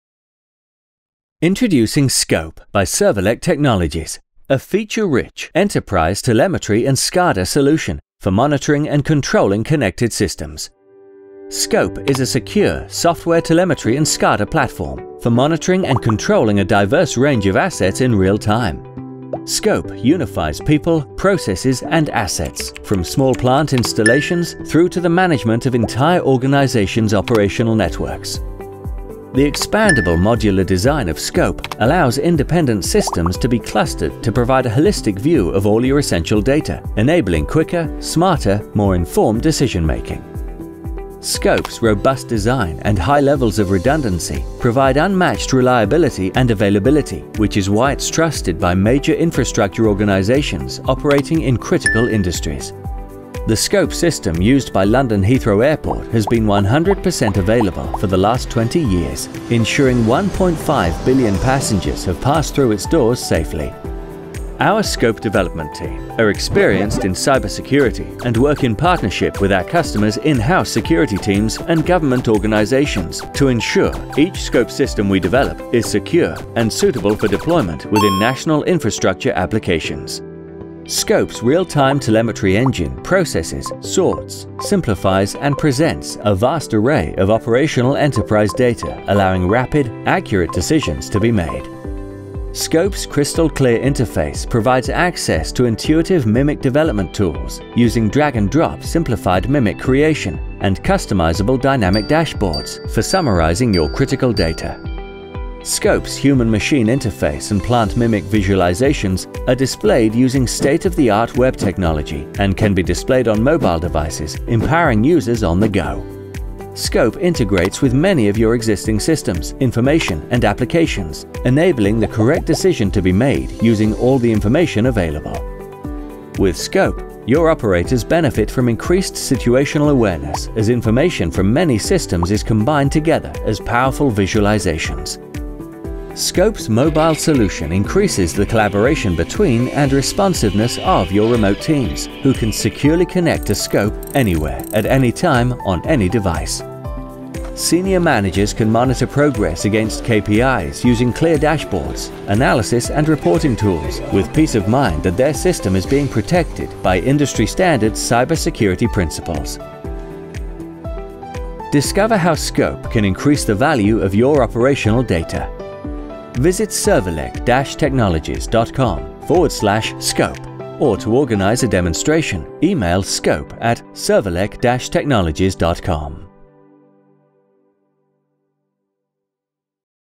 SCOPE Telemetry and SCADA Platform
SCOPE is a secure software telemetry and SCADA platform for monitoring and controlling a diverse range of assets in real-time. SCOPE unifies people, processes and assets from small plant installations through to the management of entire organisations’ operational networks.